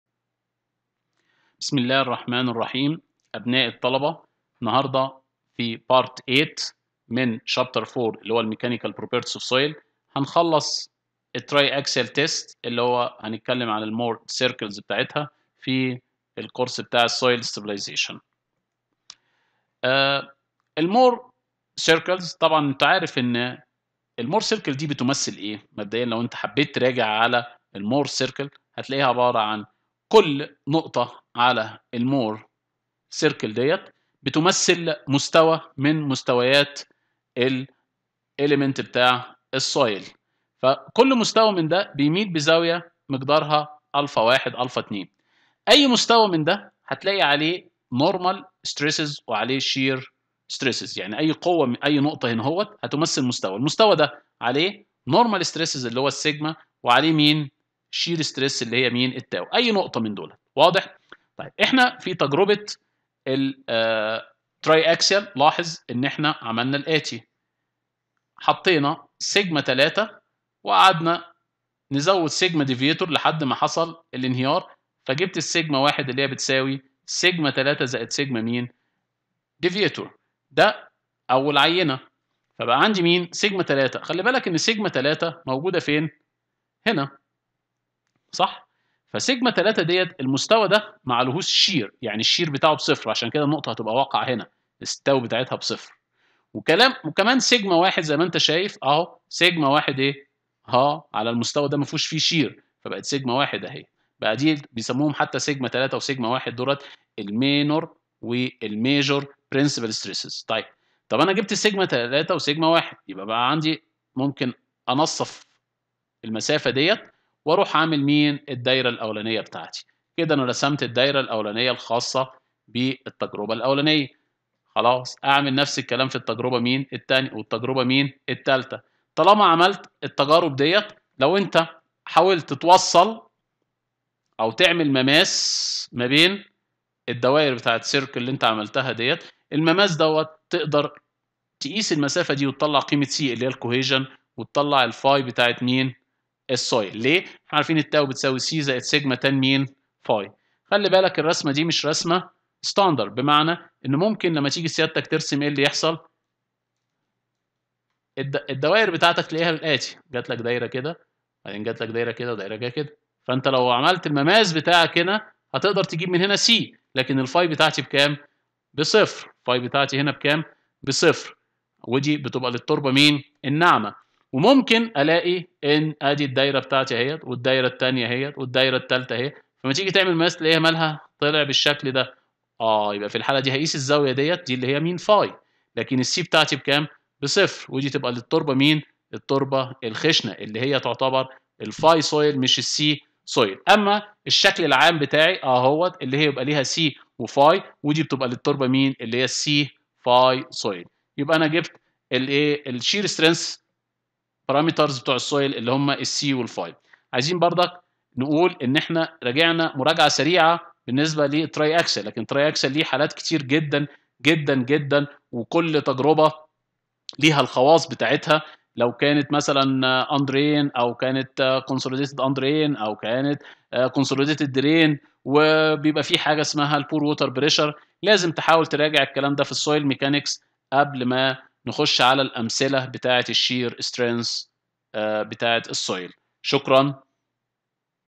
Soil Stabilization | C4-L8 | Triaxial test - 2
Chapter 4 :  Mechanical properties of soil
Lecture 8  : Triaxial test - 2
Illustration of Mohr Circles in triaxial test